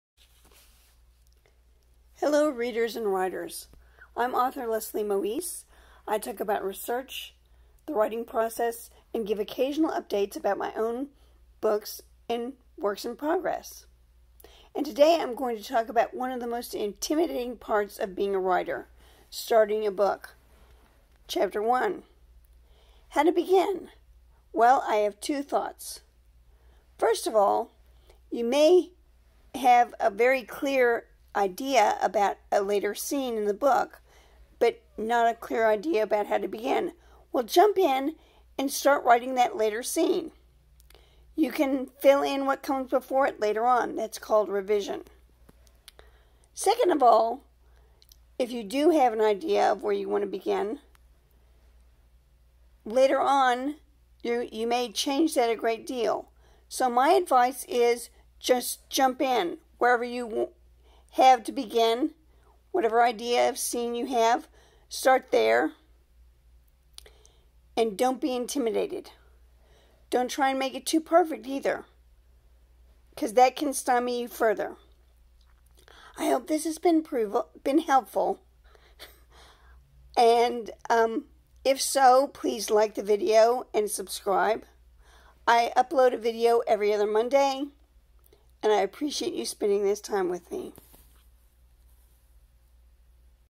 The importance of first chapters.
How to get started on a strong first chapter.